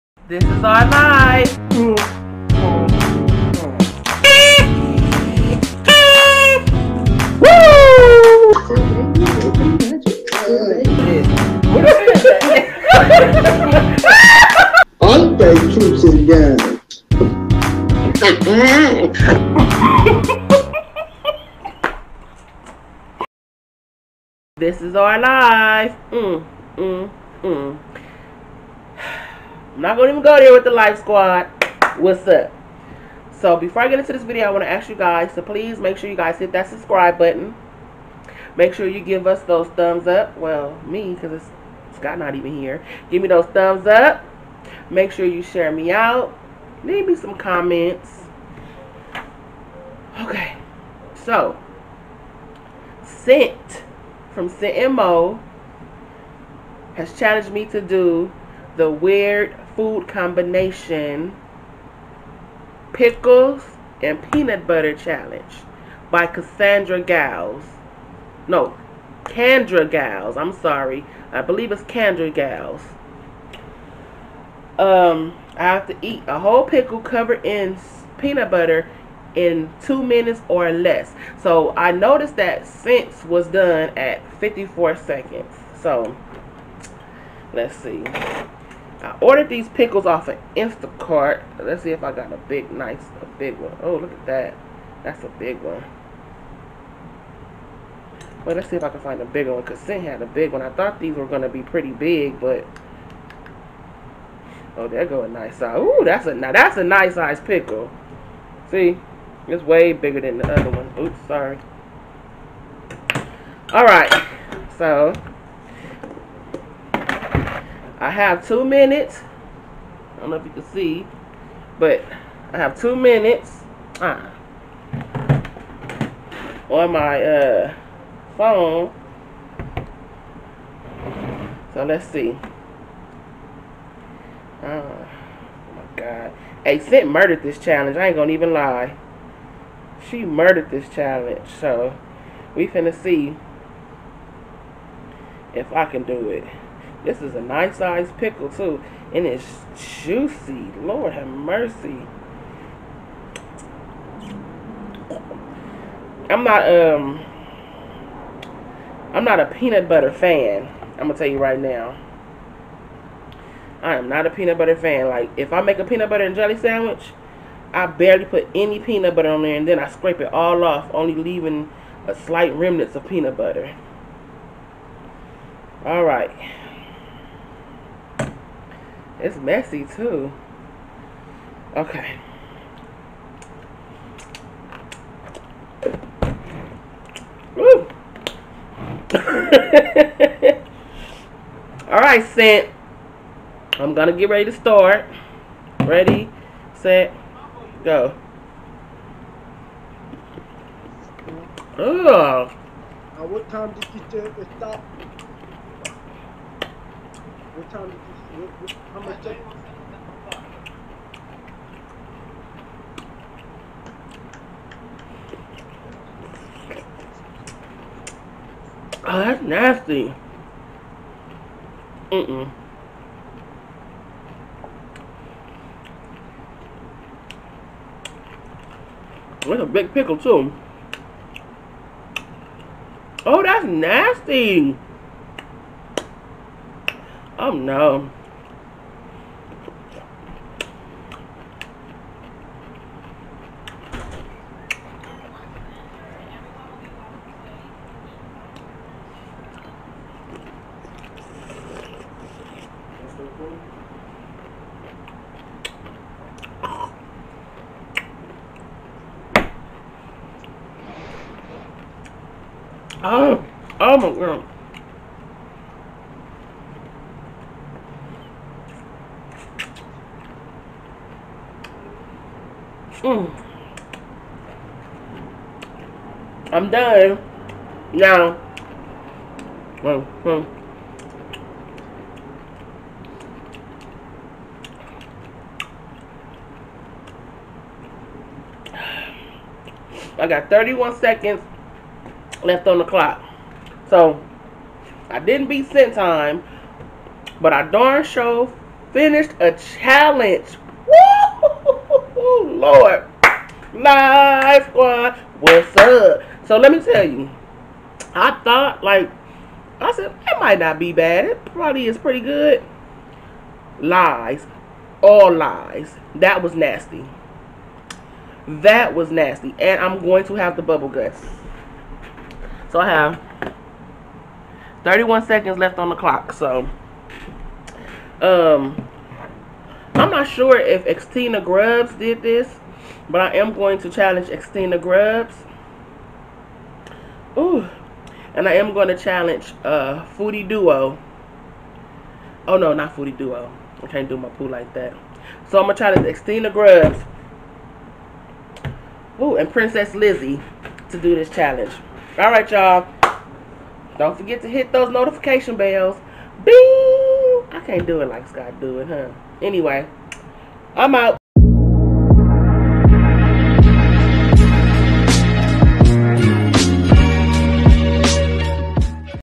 Pickle & Peanut Butter Challenge By Candra Giles
I was called out by Cynt from @CyntNMo to do this challenge and Lawd was it nasty lol